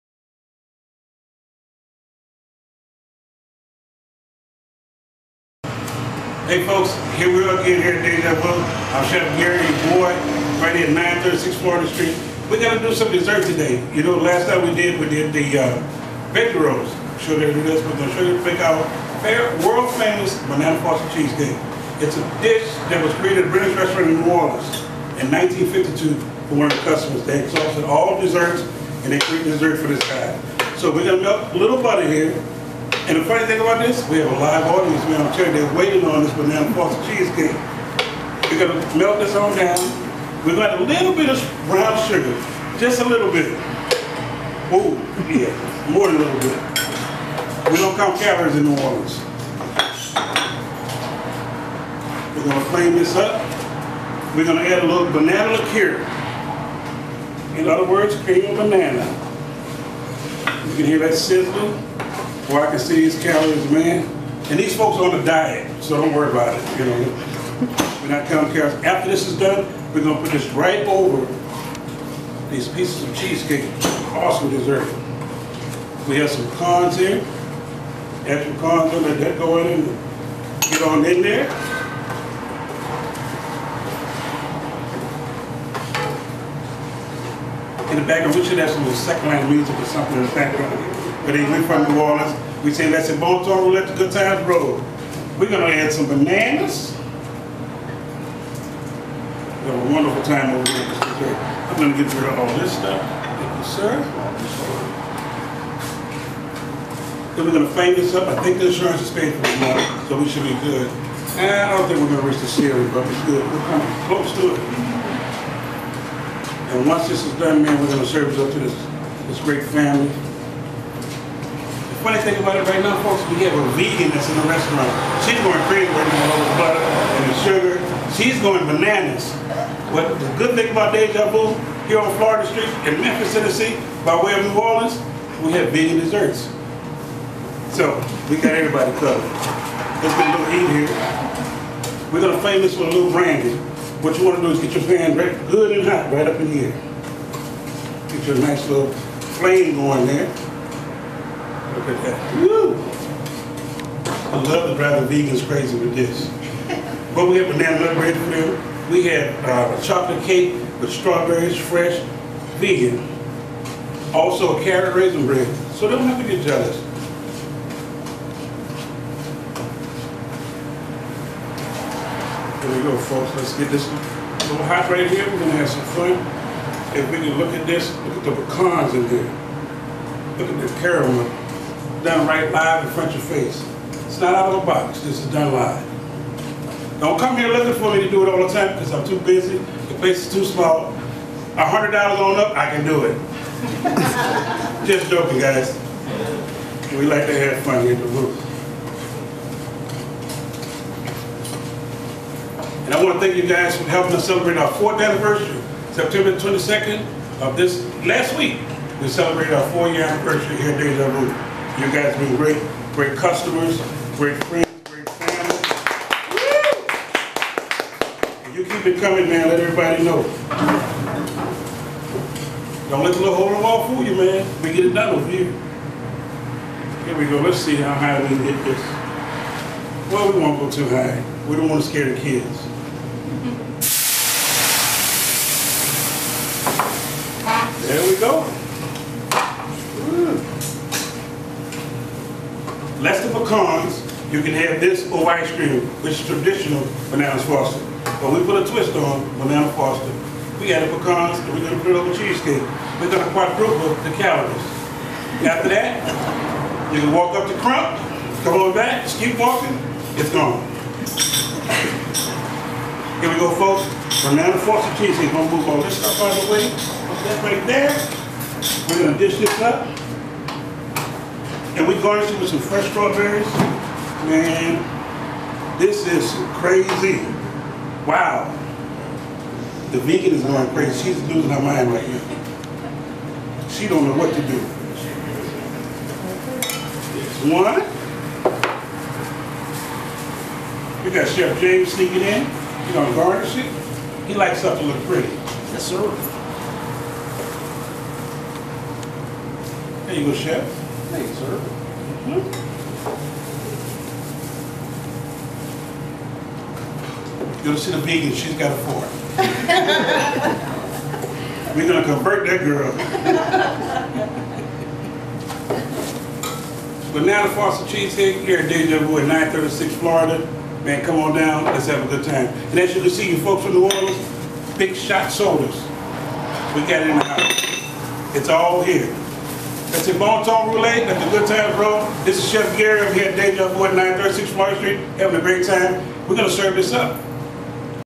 Bananas Fosters Sauce
Chef Gary always keeps it hott; he is flaming up  Bananas Foster Sauce, for his Famous Cheesecake.